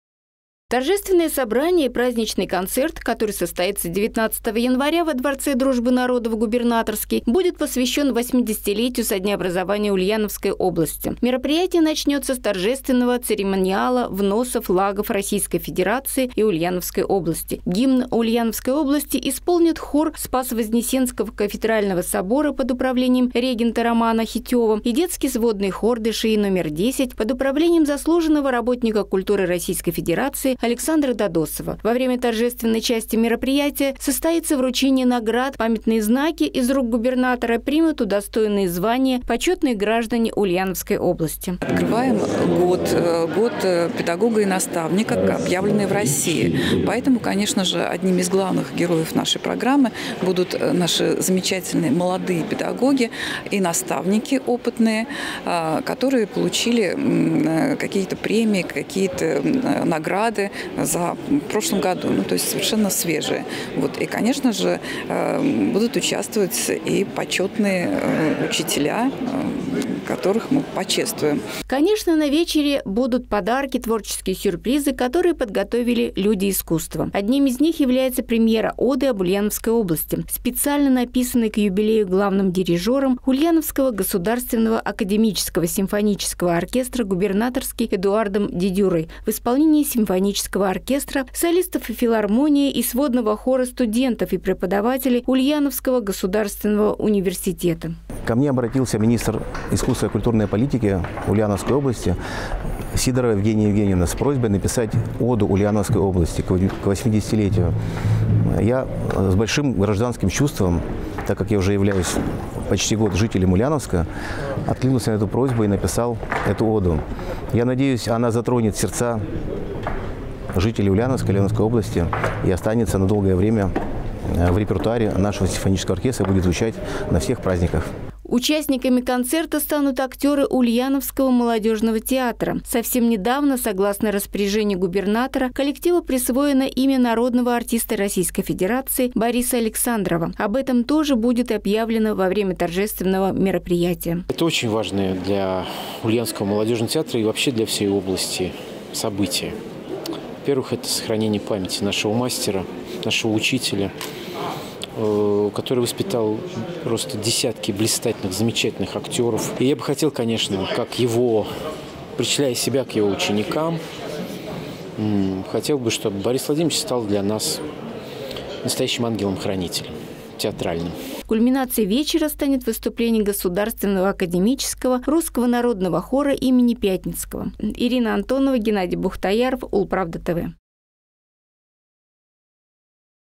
Премьера оды и хор Пятницкого. Какой концерт готовится к 80-летию Ульяновской области
19 января Ульяновская область отметит свой юбилейный день рождения. Какое мероприятие станет главным в этот день, где оно состоится и что увидят зрители – в нашем сюжете.